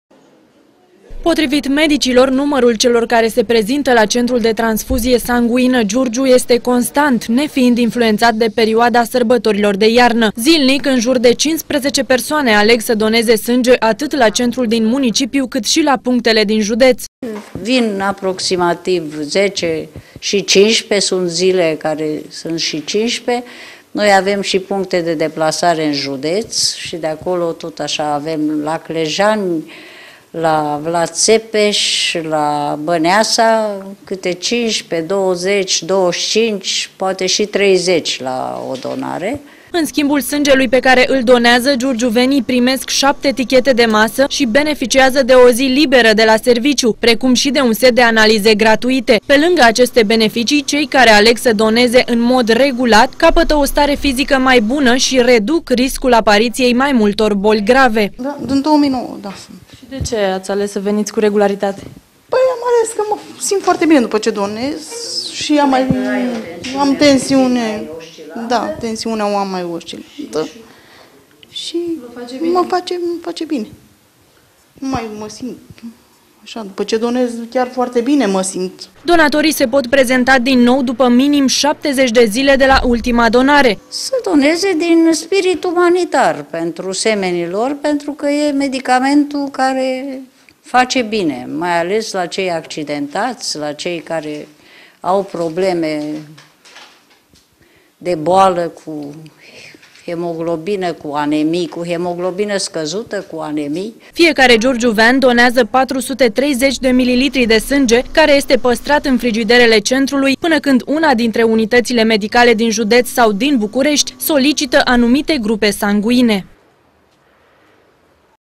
BENEFICIILE DONATORILOR
Indiferent de anotimp, giurgiuvenii se îndreaptă cu regularitate către Centrul de Transfuzie Sanguină, salvând în acest fel pe cei aflaţi în suferinţă. În schimbul sângelui pe care îl donează, aceștia primesc tichete de masă și o zi liberă de la serviciu, însă nu sunt singurele lucruri care îi atrag, ci și faptul că se simt mult mai bine după ce donează.